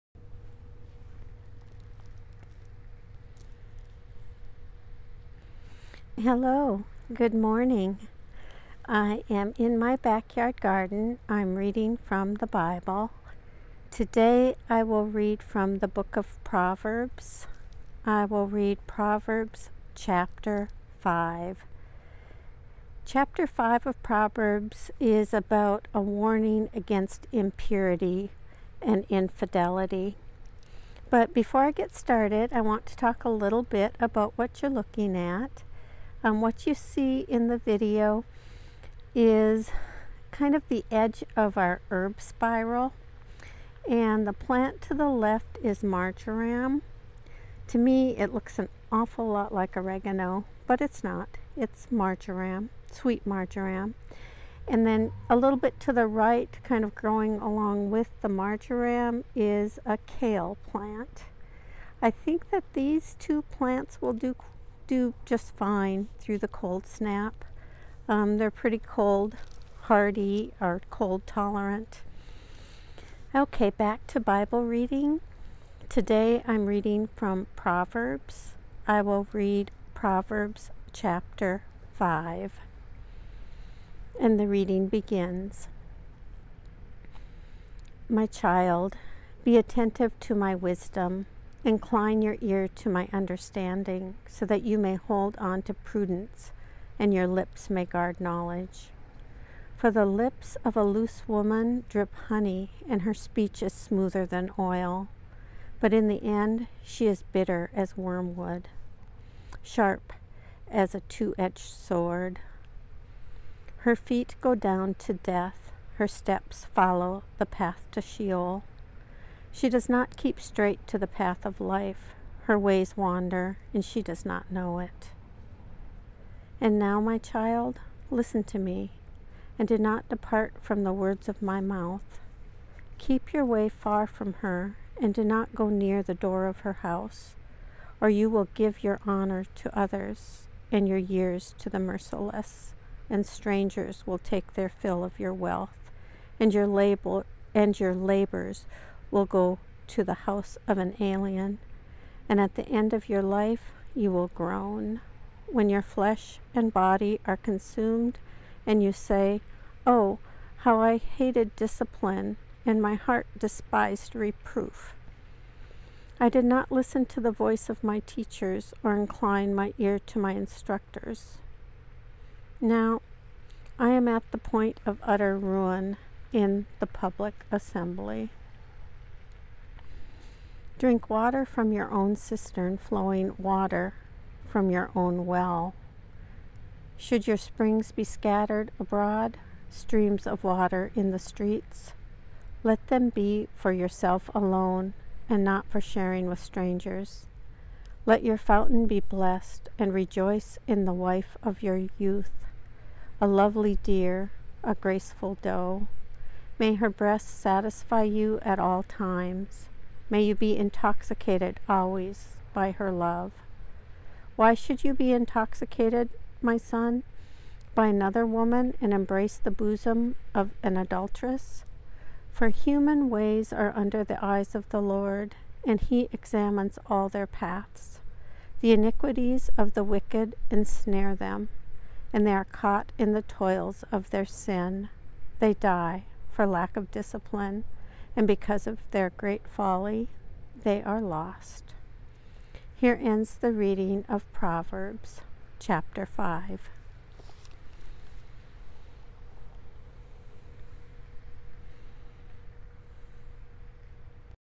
Proverbs Chapter 5 | NRSV Bible
Bible Reading | Proverbs 5 | NRSV
Reading the Bible in my Backyard Garden
Daily Bible-Based Devotional
PROVERBS CHAPTER 5
Proverbs of Solomon

00:00 Intro
00:32 About the Garden
01:24 Warning Against Impurity and Infidelity
The Holy Bible, New Revised Standard Version, NRSV, Augsburg Fortress